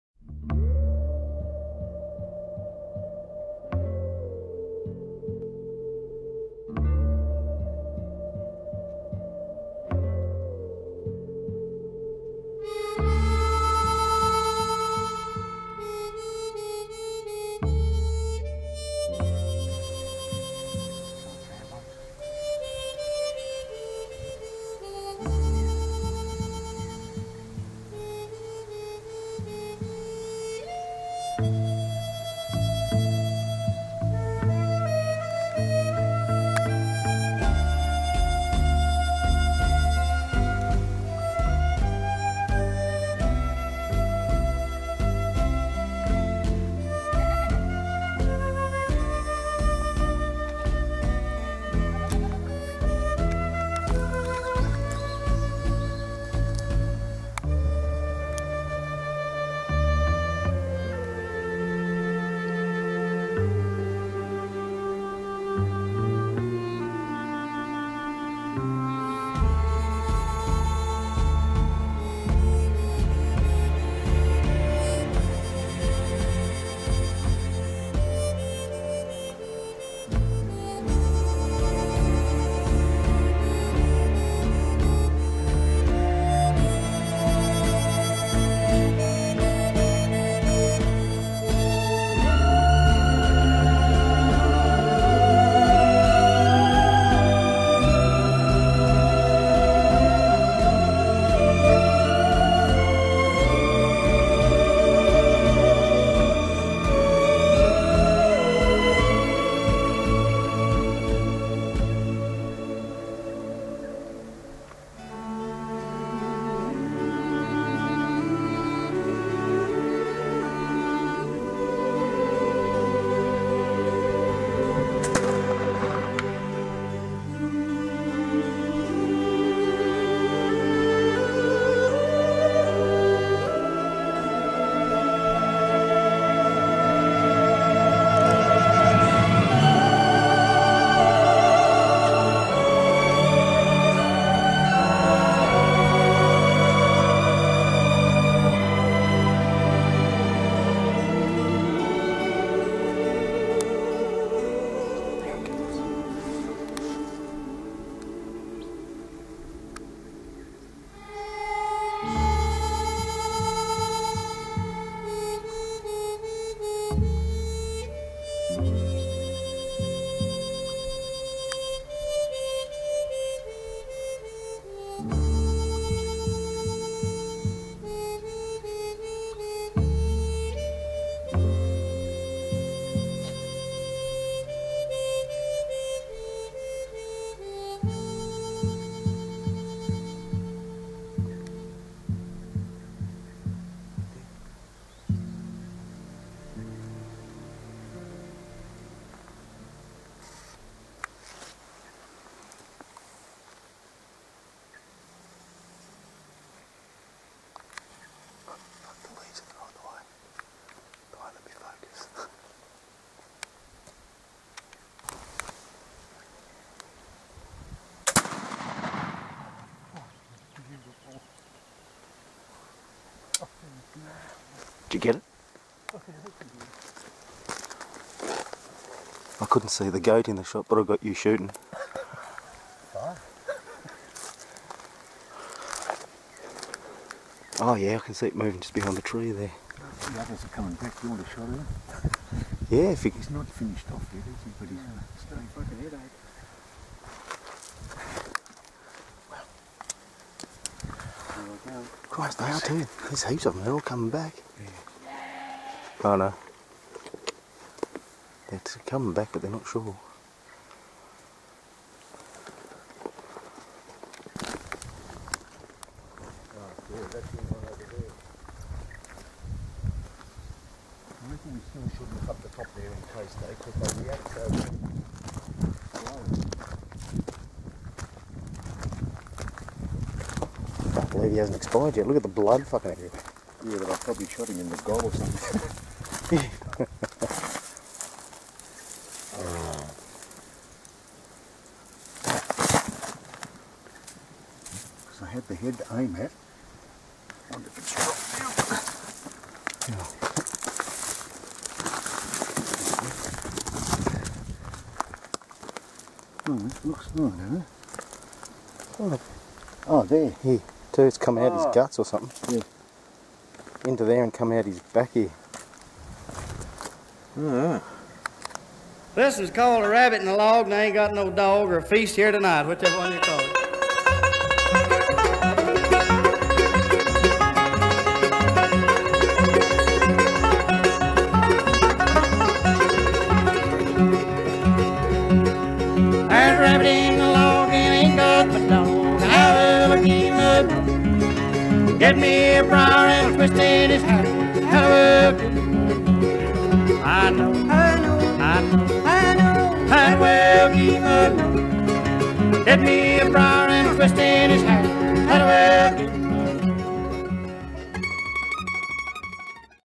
Black powder ambush
Dad shooting his 54 cal black powder gun. Its a pedersolli (cant think og the model name, Ill updae with that info when I speak to him next).
Our mate "the Outlaw" is with us to see what the gun is like.